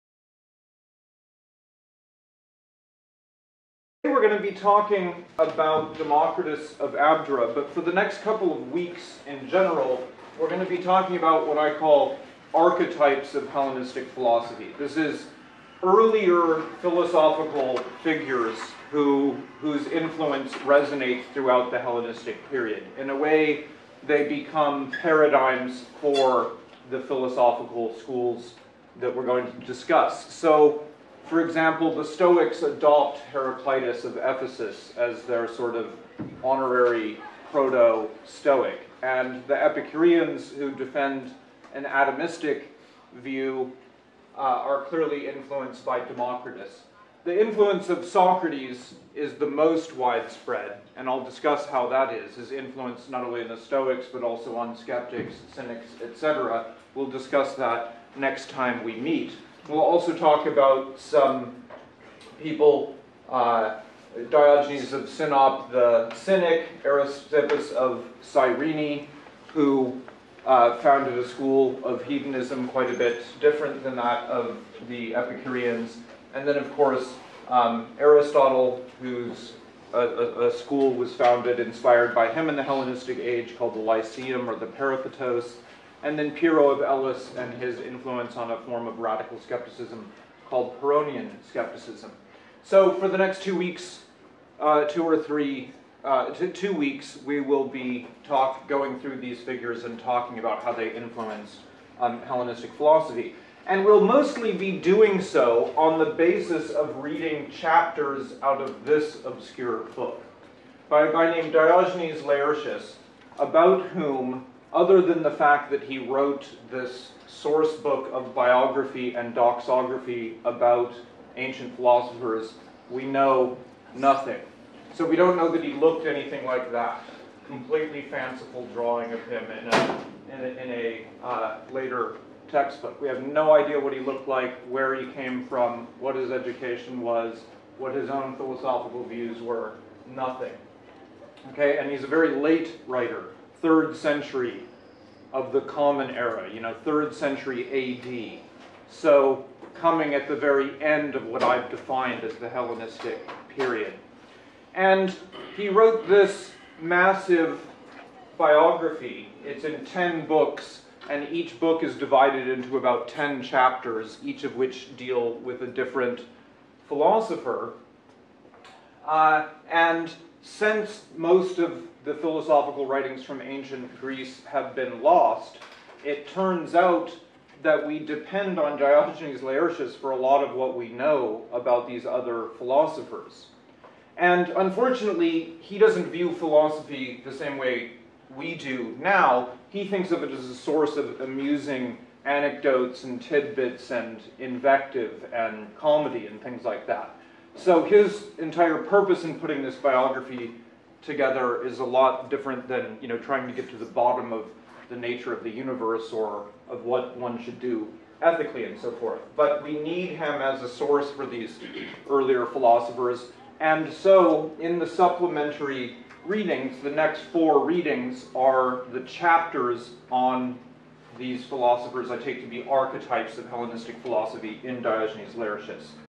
Hellenistic Philosophy 2.1.1 Archetypes of Philosophy in Diogenes Laertius
Brief introduction to Diogenes Laertius as a source of information on ancient Greek philosophy with an emphasis on his importance to Hellenistic Philosophy.